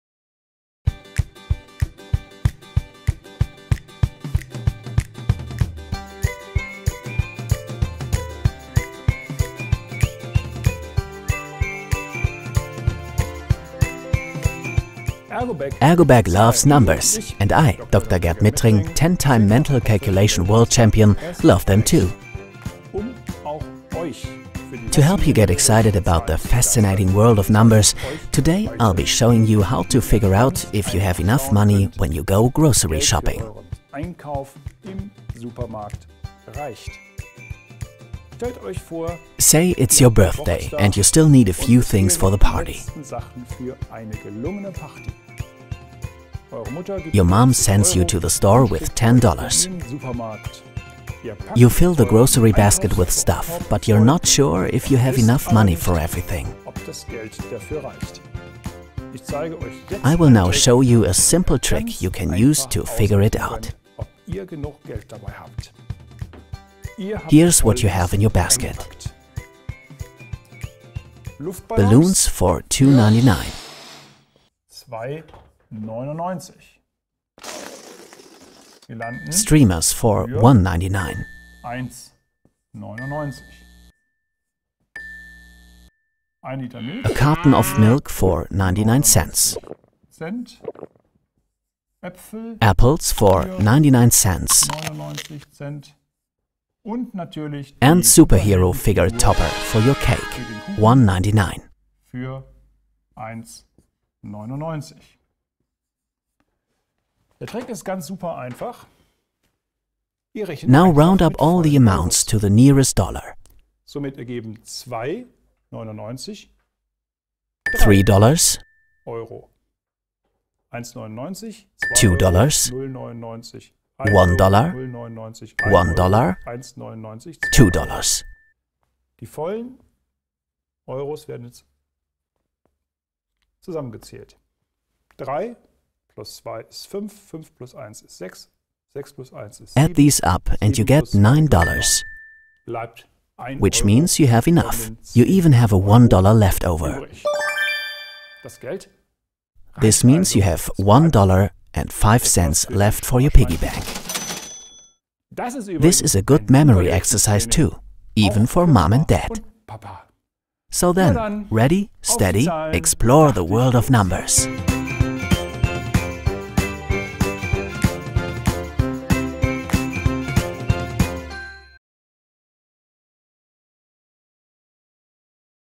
Math Tricks with Dr. Dr. Gert Mittring - Rounding up figures
"ergobag loves numbers".
Math Trick number 4: 10-time mental calculation world champion, Dr. Dr. Gert Mittring shows you in this video how to figure out if you have enough money when you go grocery shopping.